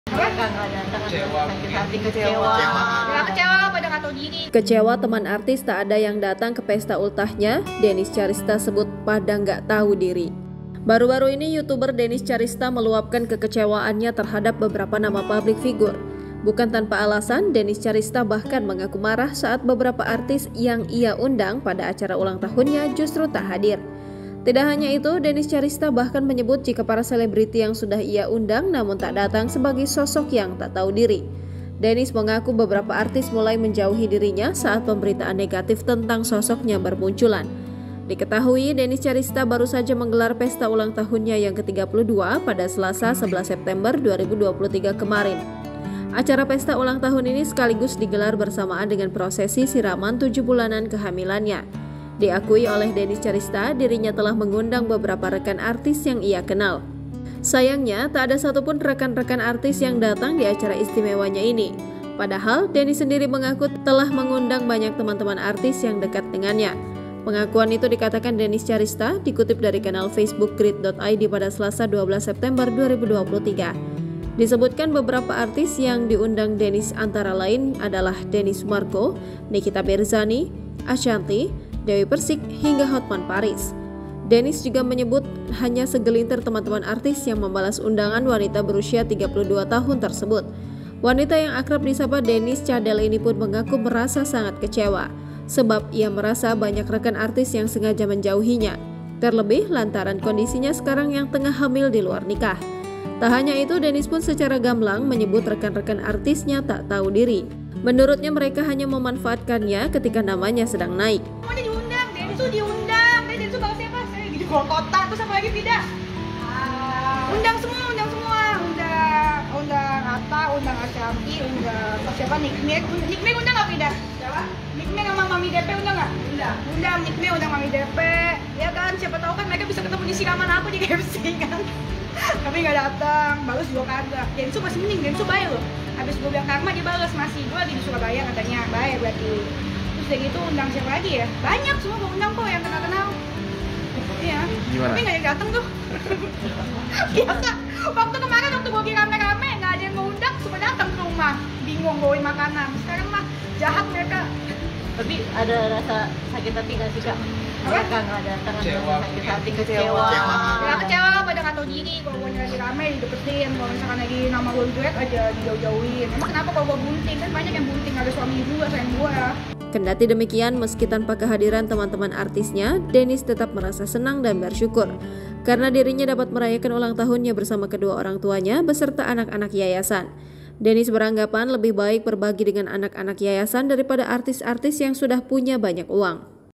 Kecewa Teman Artis Tak Ada yang Datang ke Pesta Ultahnya, Denise Chariesta: Pada Nggak Tahu Diri!
Baru-baru ini, artis Denise Chariesta meluapkan kekecewaanya terhadap beberapa nama publik figur.

Bukan tanpa alasan, Denise Chariesta bahkan mengaku marah saat beberapa artis y ang ia undang pada acara ulang tahunnya justru tak hadir.

Tidak hanya itu, Denise Chariesta bahkan menyebut jika para selebriti yang sudah ia undang namun tak datang sebagai sosok yang tak tahu diri.

Denise mengaku beberapa artis mulai menjauhi dirinya saat pemberitaan negatif tentang sosoknya bermunculan.

TribunSumsel/Euis
Editor/Nando